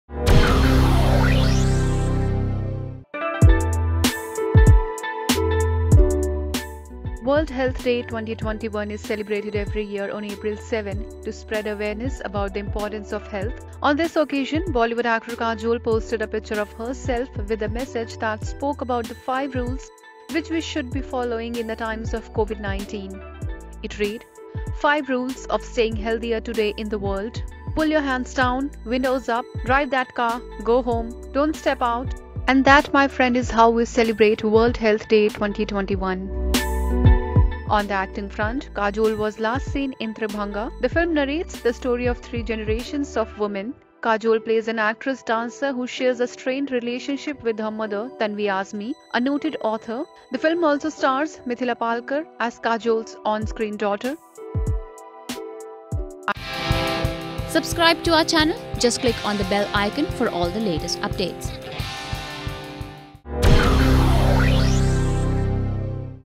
Kajol shares 5 rules of staying healthier on World Health Day
World Health Day 2021 is celebrated every year on April 7 to spread awareness about the importance of health.